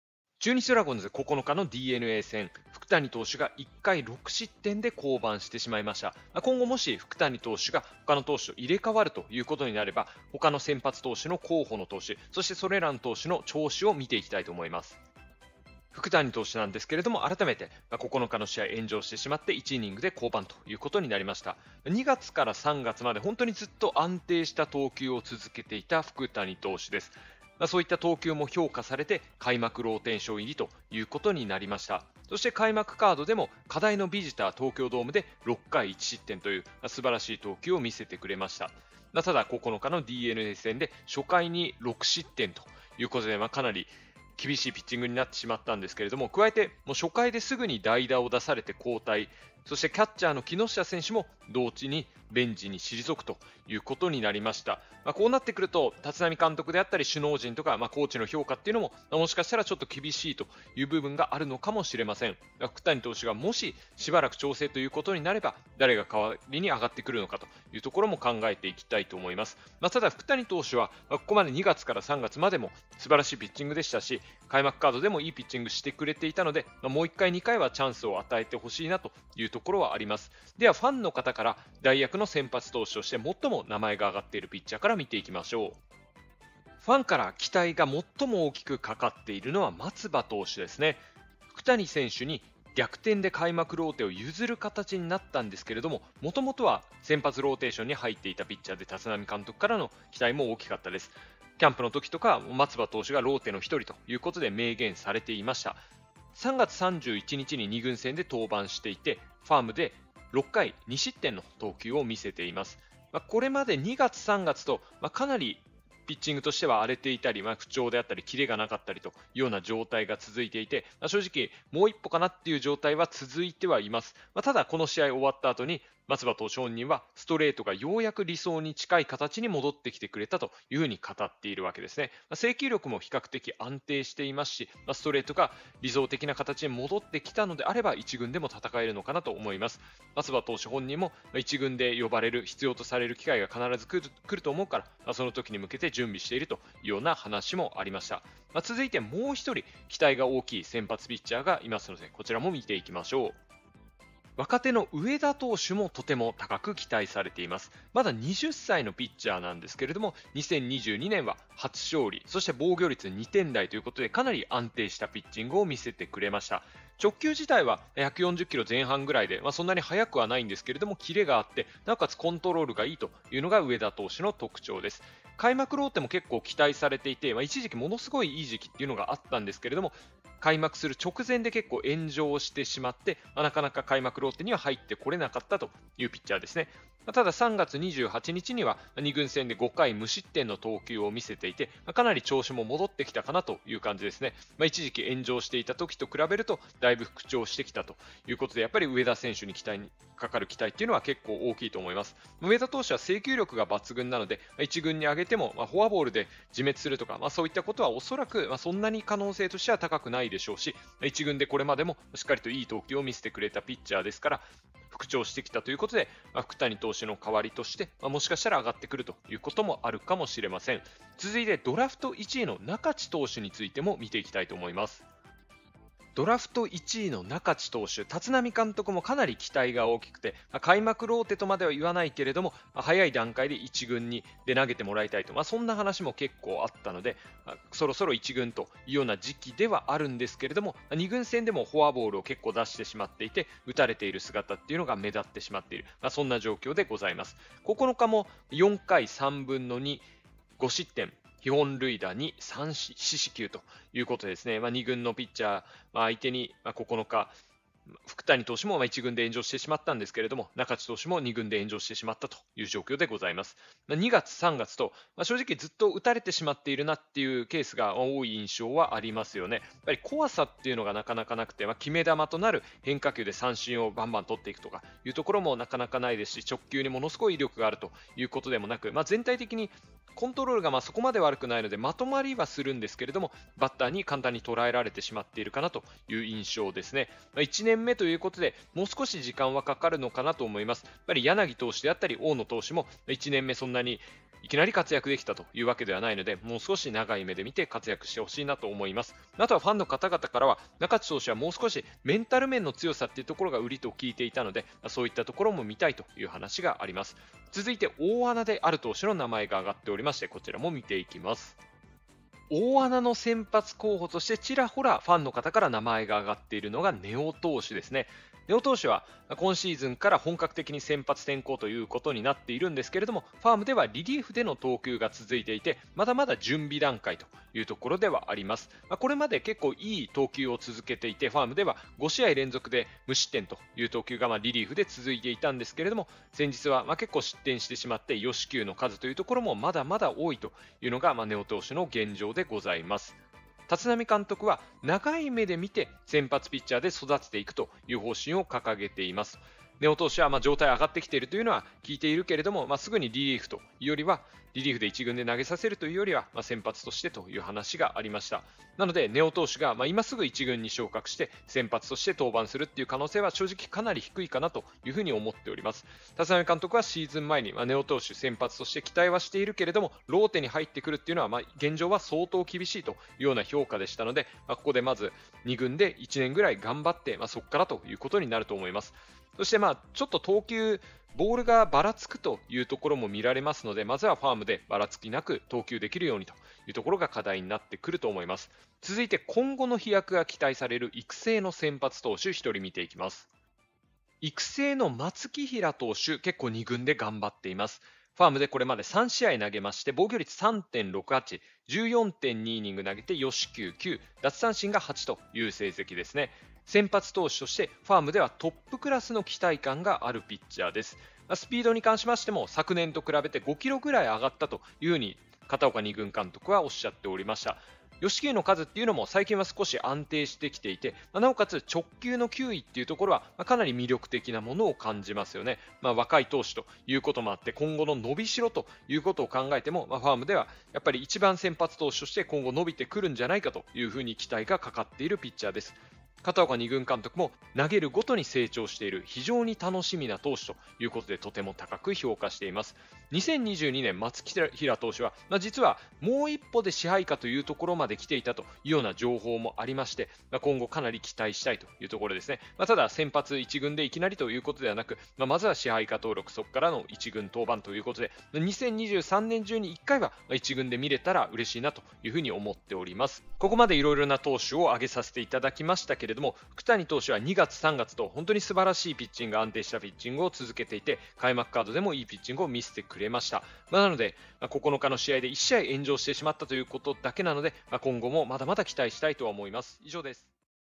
中日 福谷が初回で降板させられる。２軍の先発候補５人【中日ドラゴンズ/立浪監督】
中日ドラゴンズ、福谷が１回６失点で降板。２軍でアピールする先発投手との入れ替えもあるか。立浪監督の判断は。